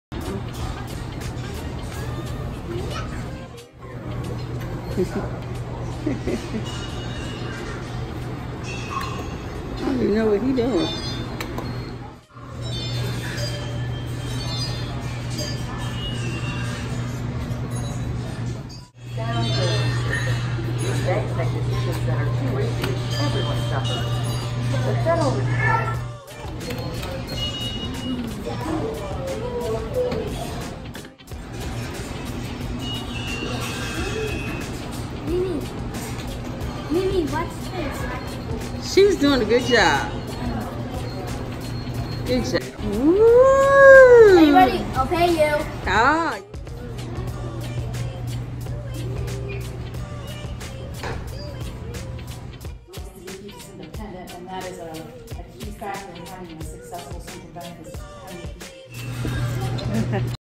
Mogul Academy Corporate Field Trip to the Federal Reserve Bank and Money Museum
Mogul Academy at the Money Museum